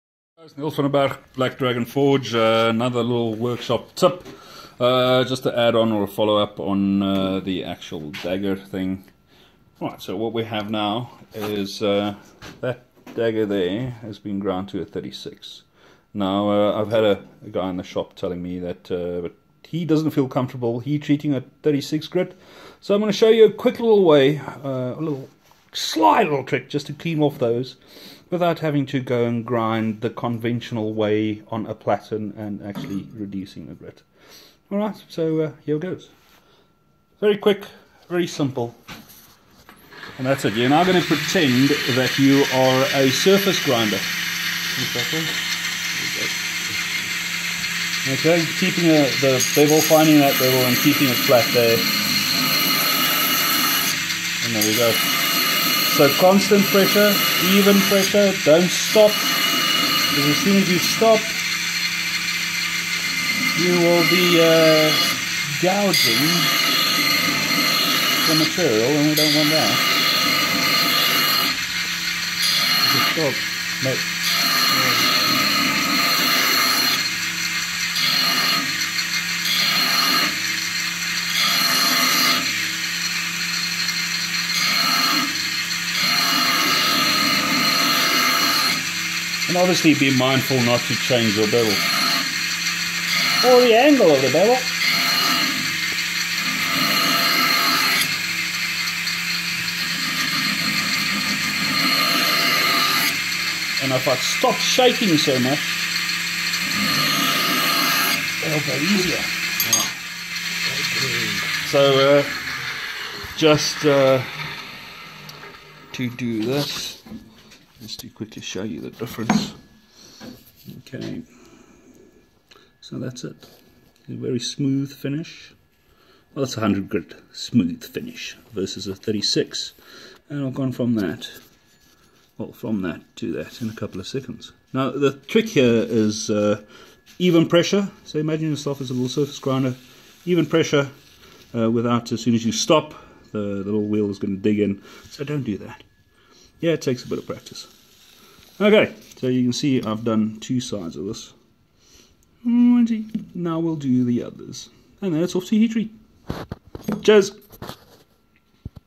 Be the surface grinder!
Quick little trick to a smooth 100 grit finish before heat treatment.